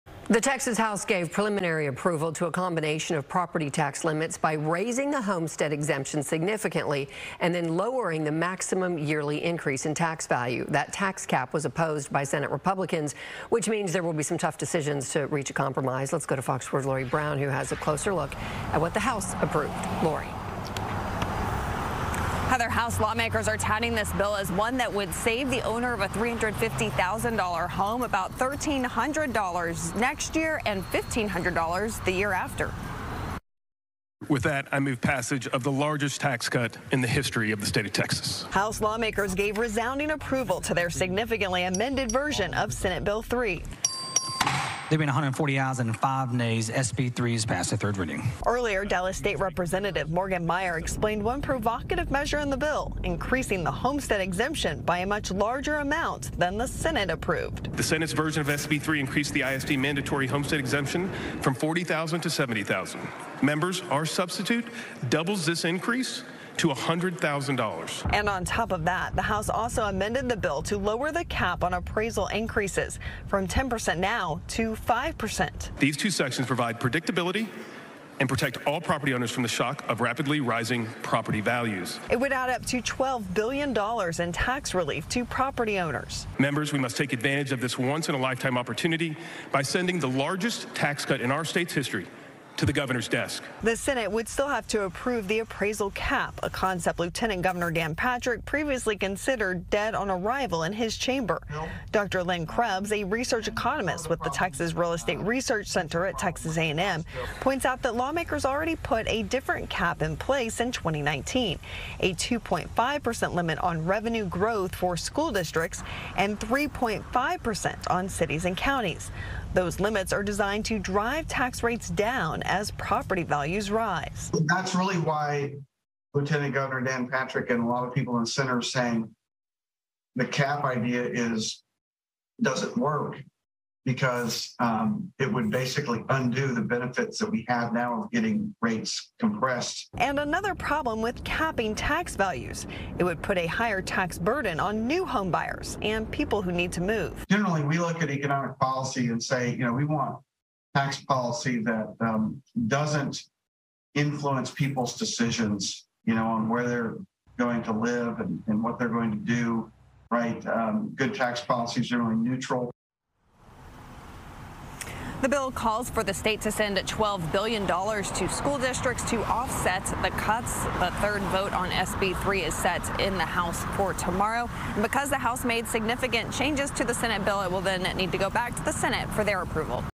Texas House considers bill raising homestead exemptions, lowering yearly tax value increases
The Texas House gave preliminary approval to a combination of property tax limits by raising the homestead exemption significantly and lowering the maximum yearly increase in tax value.